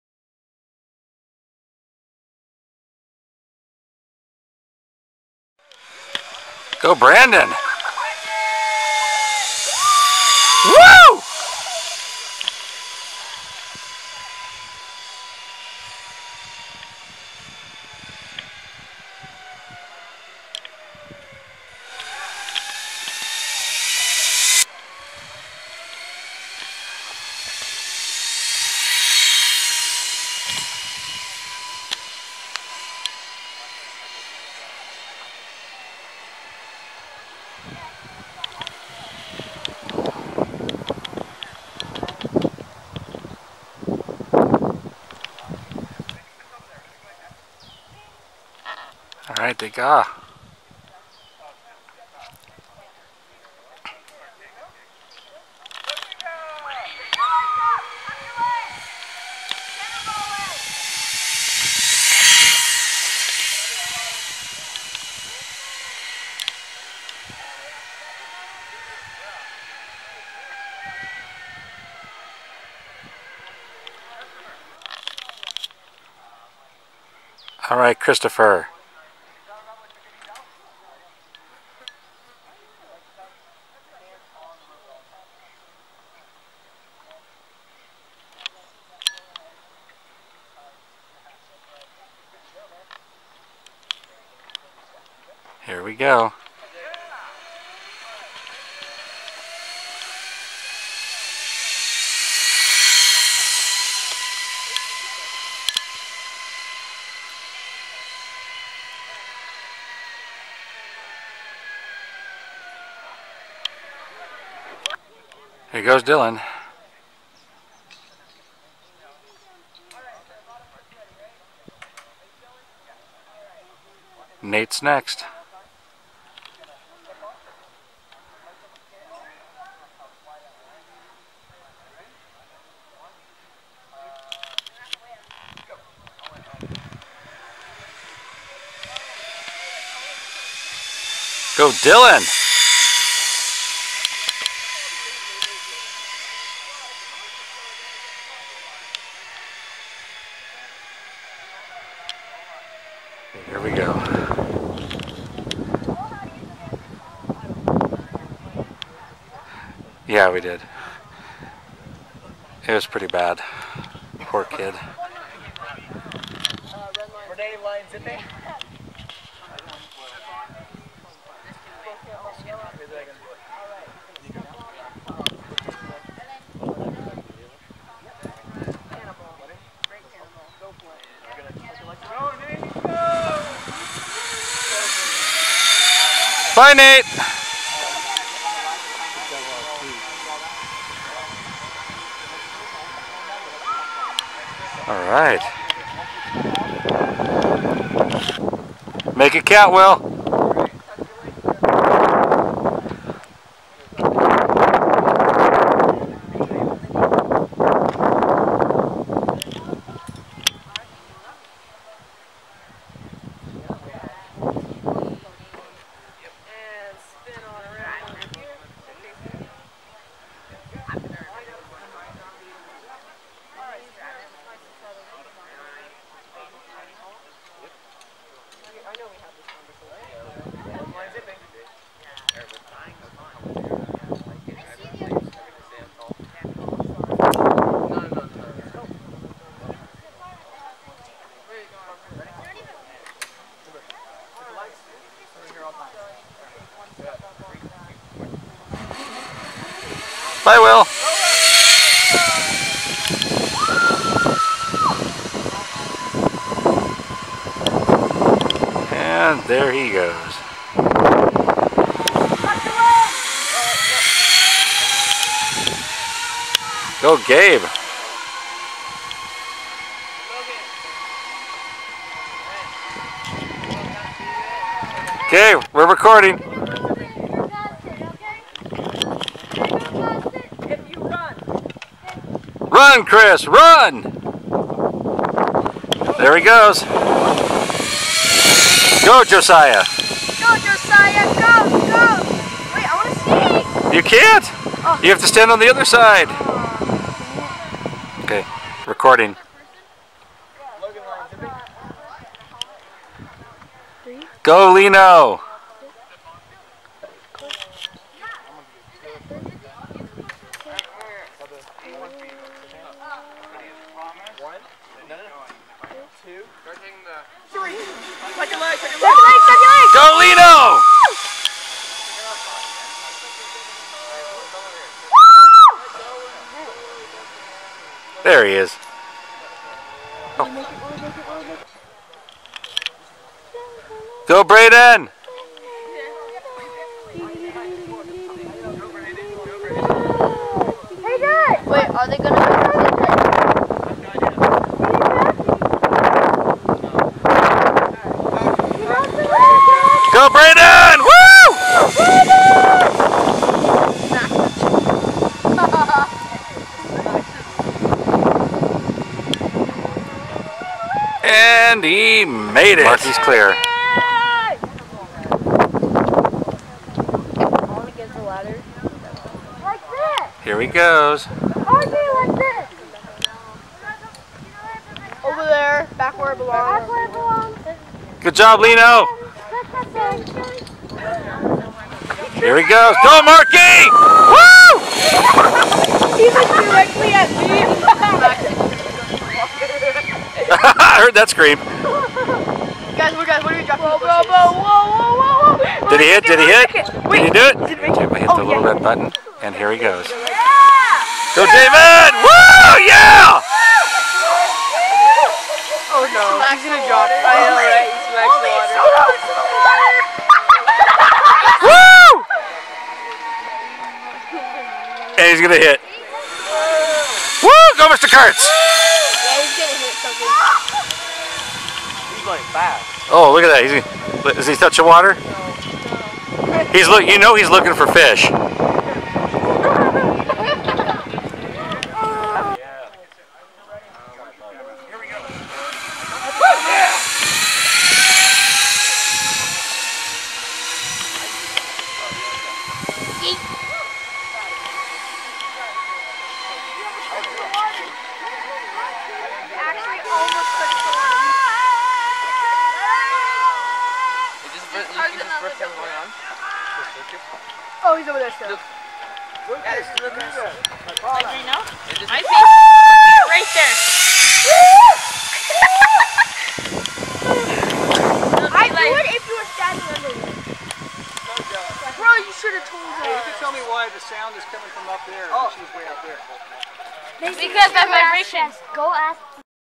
2019-7-17 Troop 66 Zip Lining at Hubert Eaton Scout Reservation
Troop 66 Zip Lining at Hubert Eaton Scout Reservation